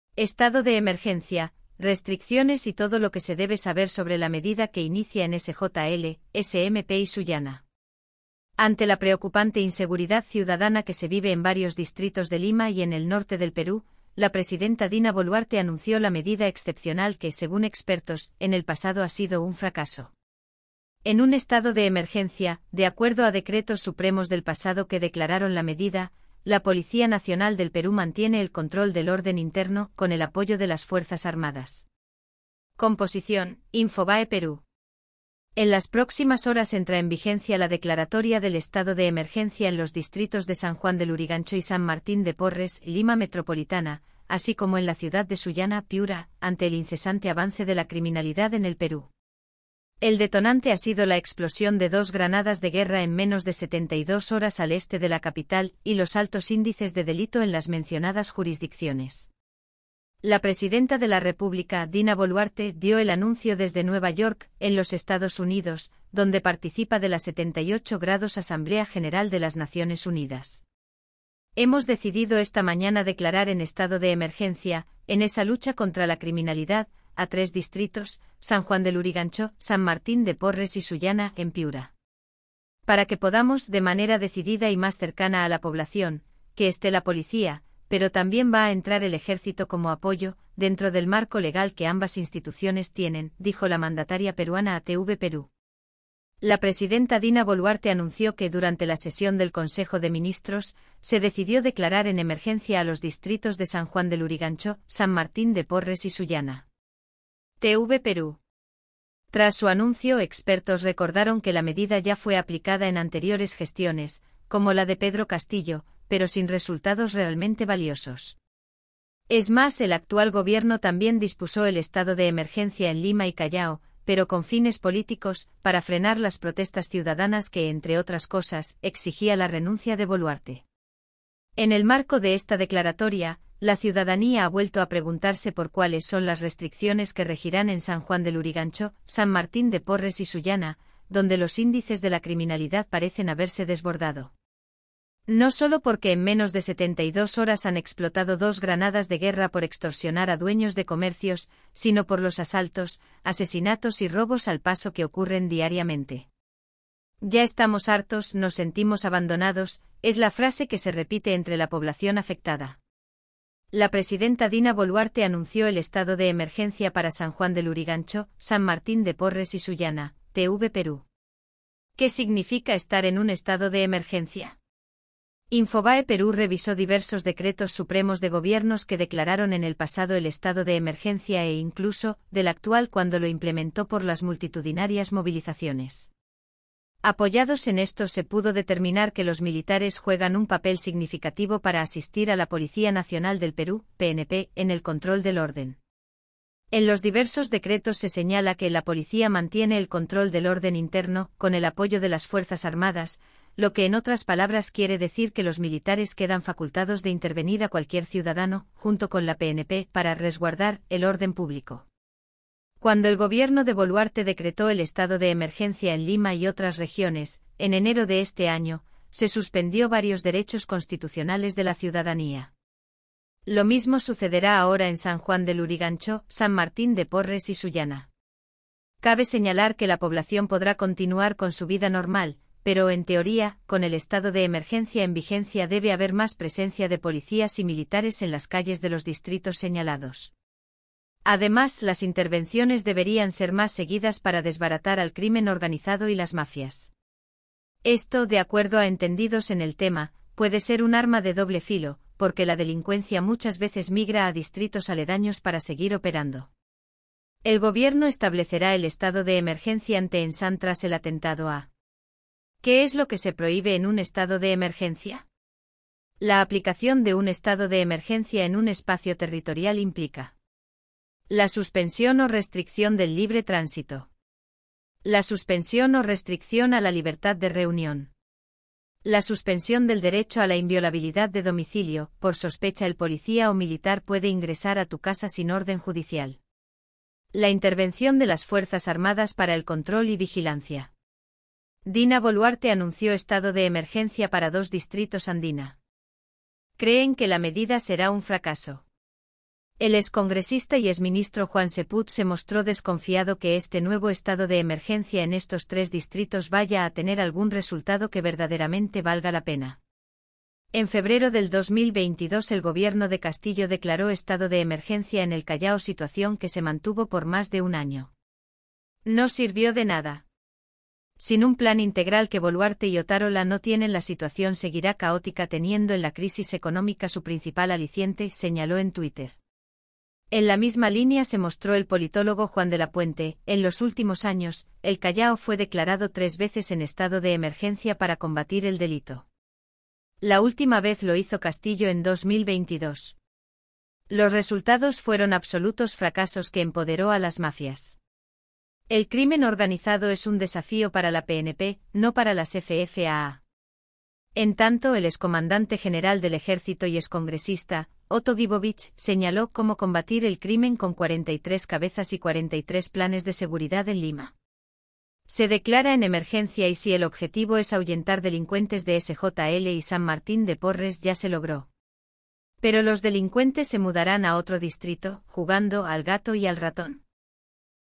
Estado de Emergencia restricciones y todo lo que se debe saber sobre la medida que inicia en SJL, S
Ante la preocupante inseguridad ciudadana que se vive en varios distritos de Lima y en el norte del Perú, la presidenta Dina Boluarte anunció la medida excepcional que, según expertos, en el pasado ha sido ‘un fracaso’.
En un Estado de Emergencia, de acuerdo a decretos supremos del pasado que declararon la medida, la Policía Nacional del Perú mantiene el control del orden interno, con el apoyo de las Fuerzas Armadas. (Composición: Infobae Perú)
En las próximas horas entra en vigencia la declaratoria del Estado de Emergencia en los distritos de San Juan de Lurigancho y San Martín de Porres (Lima Metropolitana), así como en la ciudad de Sullana (Piura), ante el incesante avance de la criminalidad en el Perú. El detonante ha sido la explosión de dos granadas de guerra en menos de 72 horas al este de la capital y los altos índices de delito en las mencionadas jurisdicciones.
La presidenta de la república, Dina Boluarte, dio el anuncio desde Nueva York, en los Estados Unidos, donde participa de la 78º Asamblea General de las Naciones Unidas.